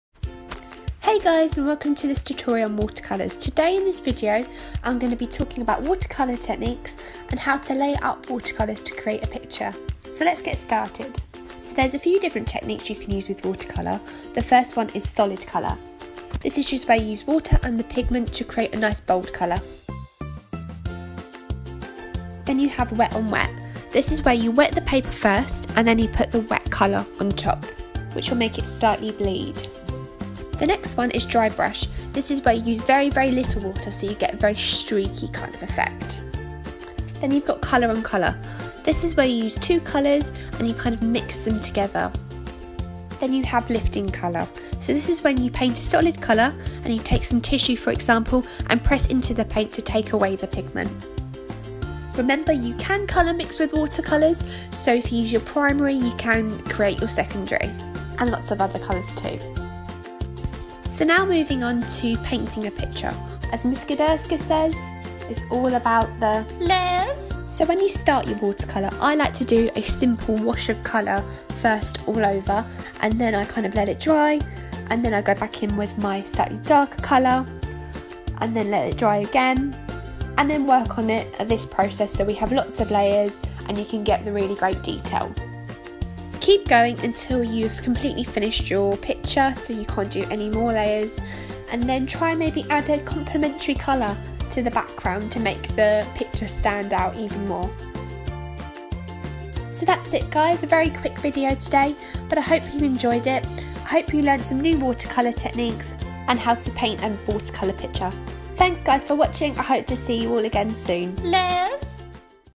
Water color techniques, and how to use them
Whitefriars School. In this video you will learn about simple water color techniques and how to layer watercolors, this is a very simple tutorial and jut some basic skills.

You will need:

Cartridge paper
A watercolour pallet
Paint brushes
Water pot with water and some tissue paper